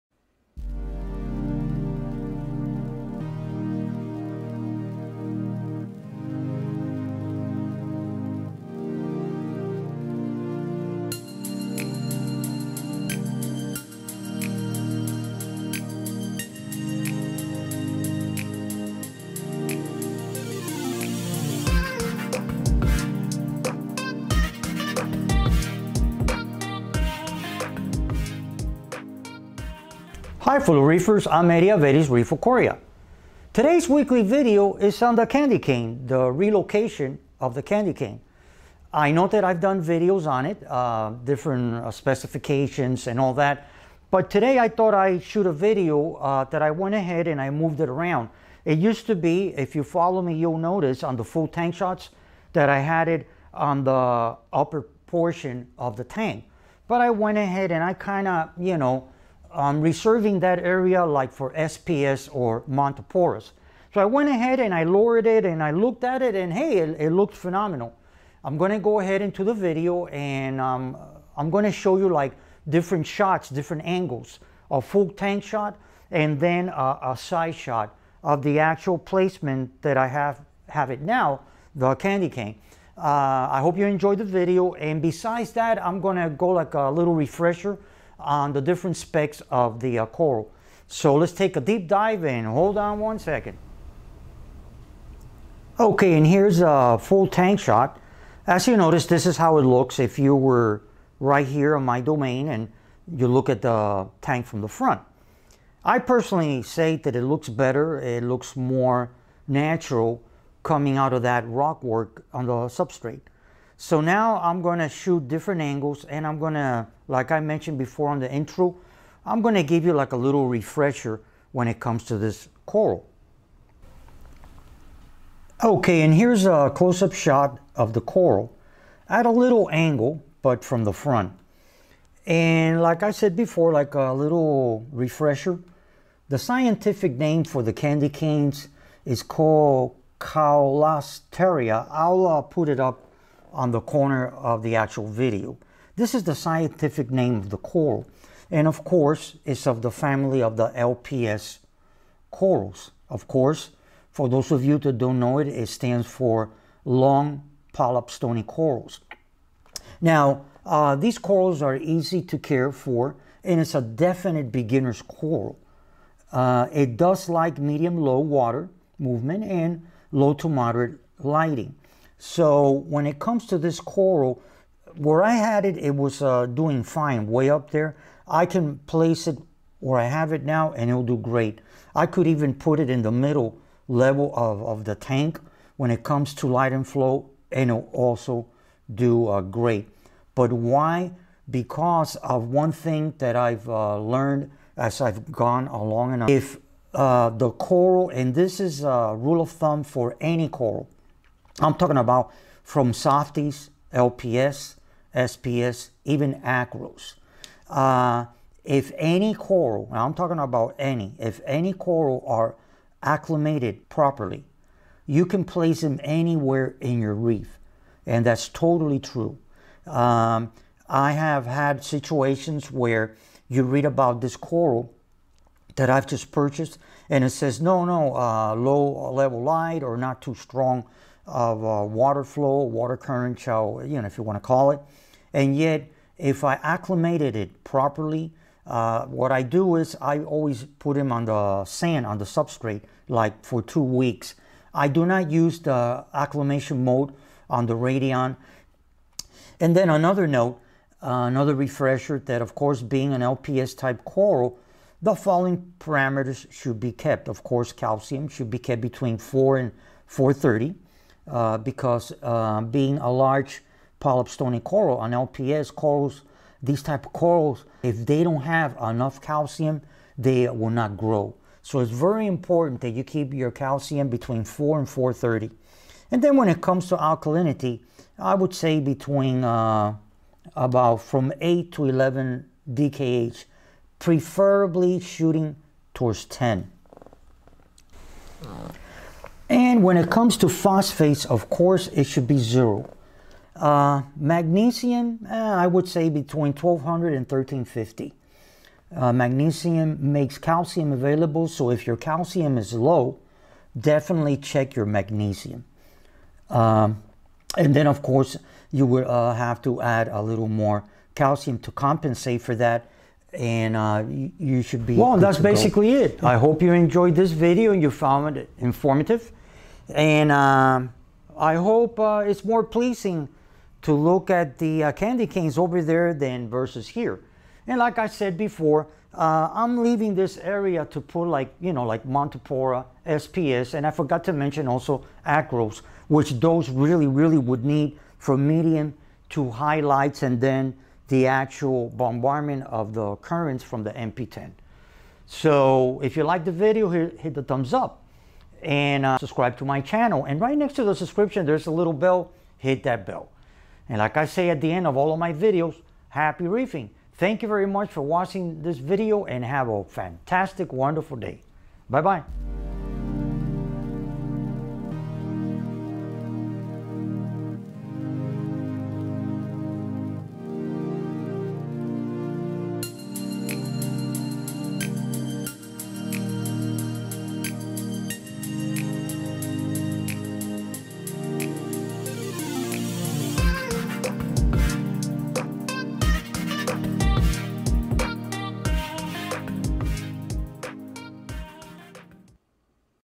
Candy Cane Coral - Relocation
This weeks video is on the relocation of the Candy Cane LPS Coral in which I discuss the new placement and a recap of caring for it.